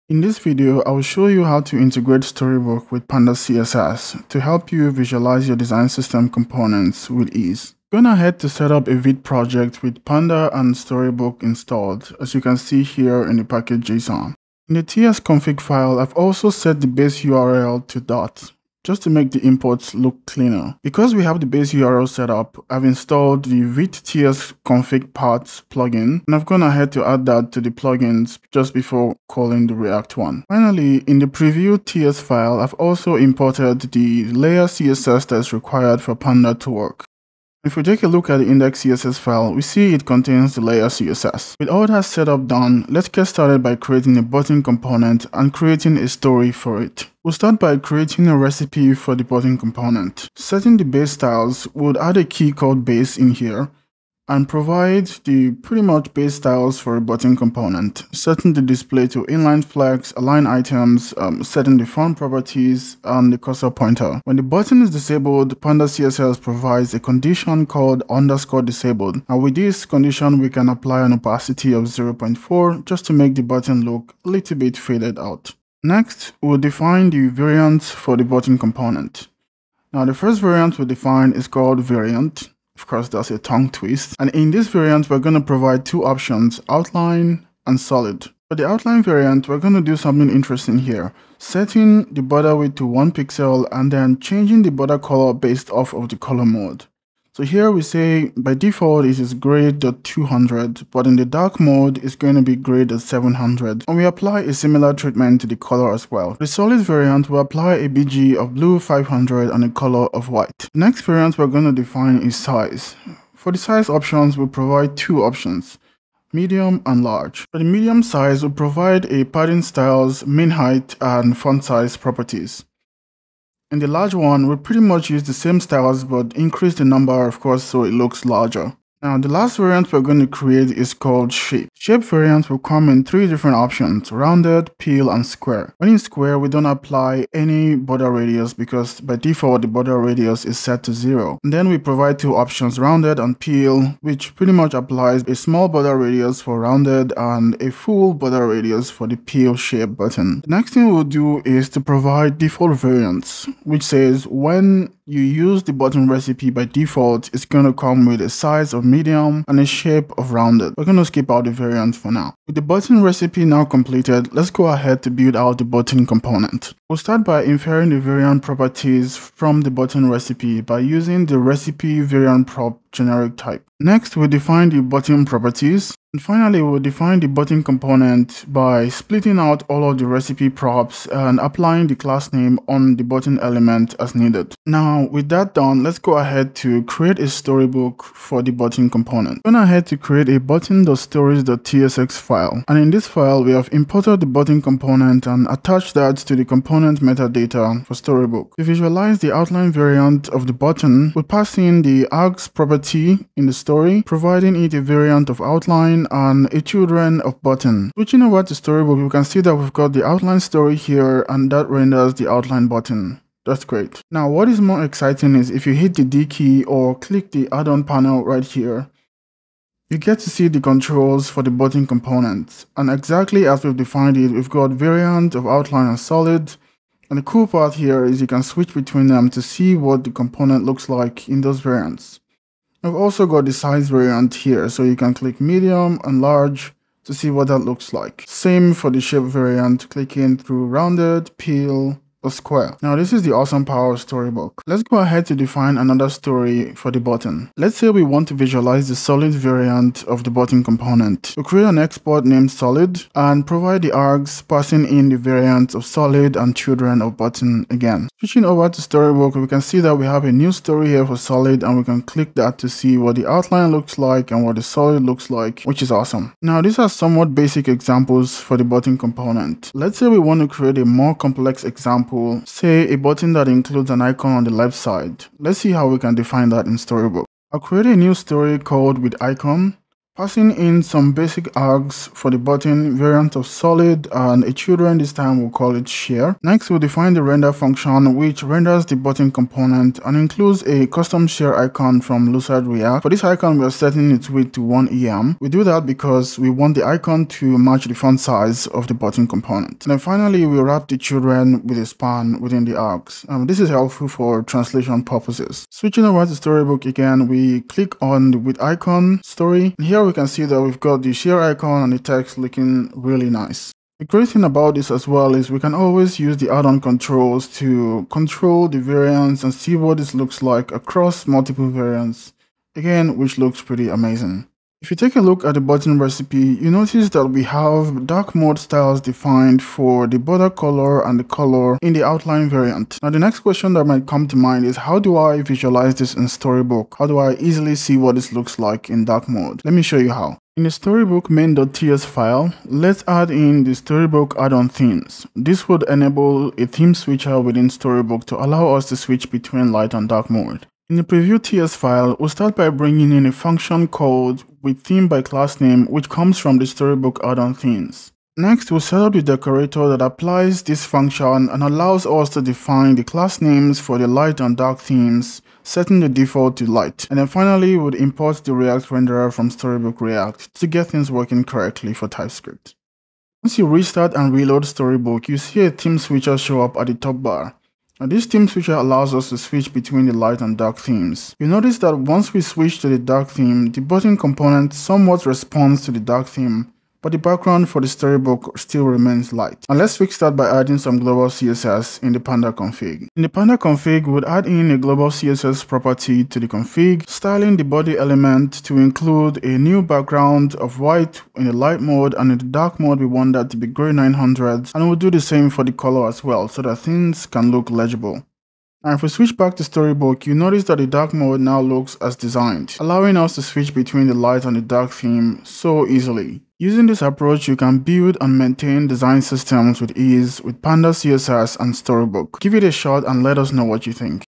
Storybook + Panda CSS (Light and Dark Mode)
Welcome to our latest video tutorial, where I show how to integrate Storybook with Panda CSS. We'll also set up storybook-addon-themes to visualize light and dark modes for a seamless development experience.

Chapters
00:00 - Setting up Storybook and Panda
00:46 - Creating a Button Recipe
03:10 - Creating the Button Stories
05:20 - Light and Dark Mode Setup